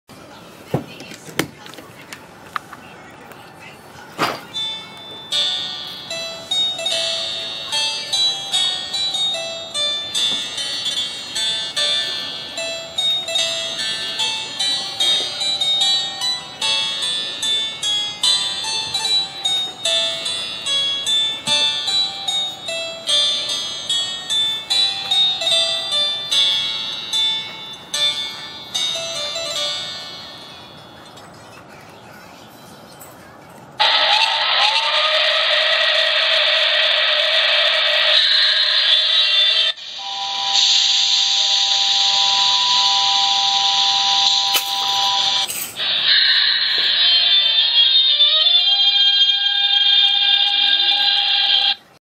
DFPlayer Mini Test
DCCデコーダ用に動作テストをしました。Arduinoからシリアル通信経由で操作し、単体動作は問題ないことを確認できました。３米ドル程度で購入できます。